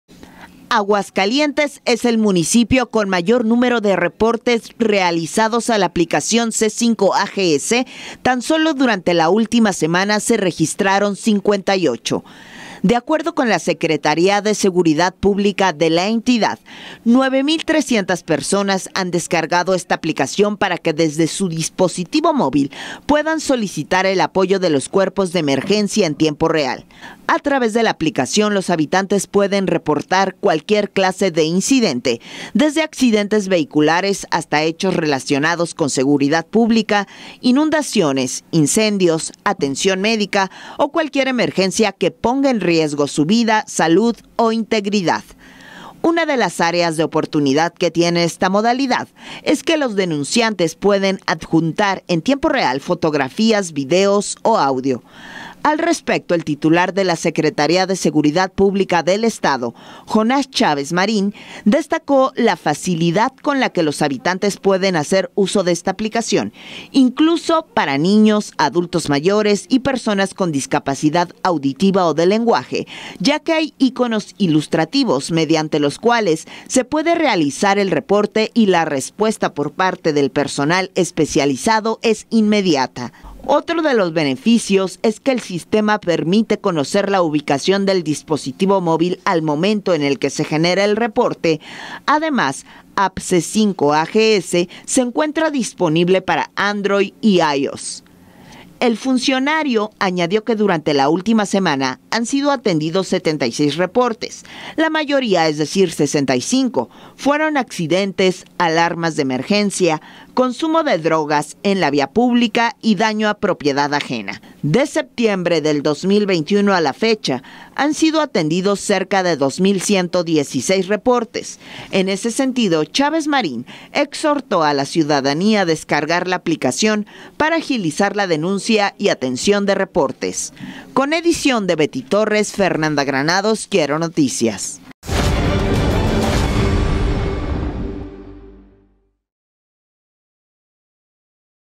Un éxito la app de auxilio
Casi 10 mil personas han descargado la app C5AGS, la cual permite a la ciudadanía realizar reportes de emergencia en tiempo real desde su dispositivo móvil.